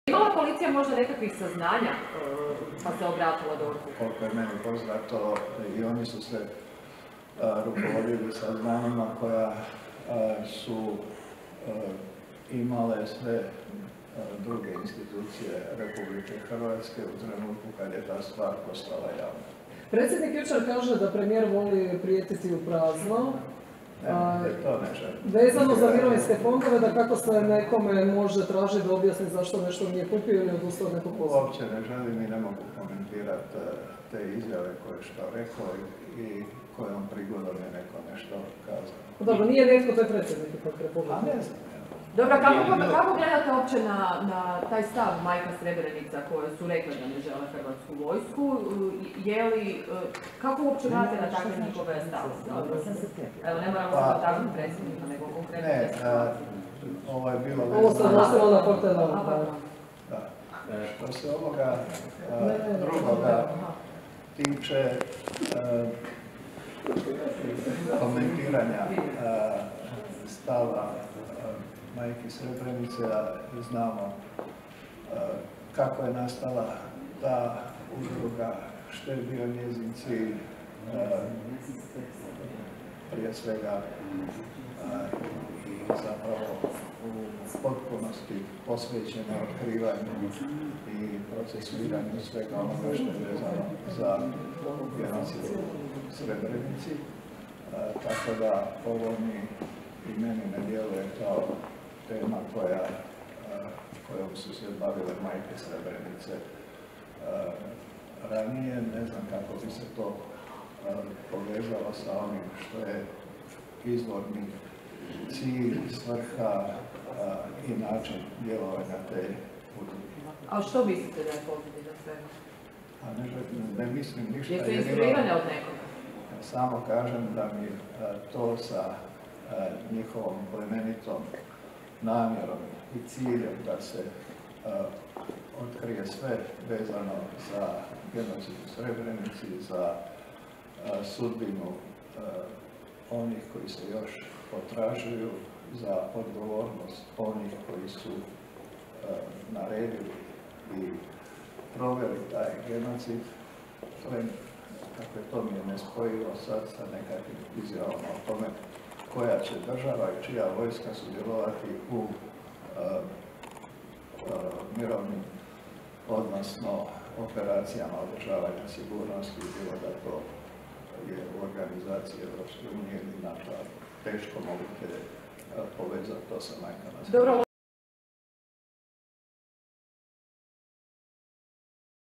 Ministar Božinović o izjavama Majki Srebrenice
Izjava ministra Davora Božinovića nakon redovite sjednice vlade.